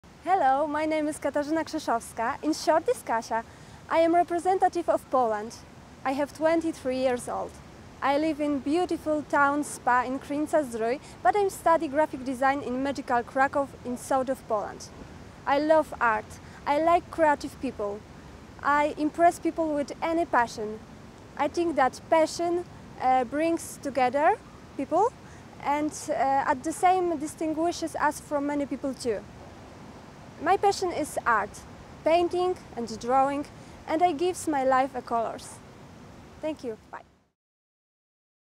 Miss World 2013 - Profile Video - Poland
Meet Katarzyna Krzeszowska, Miss Poland World!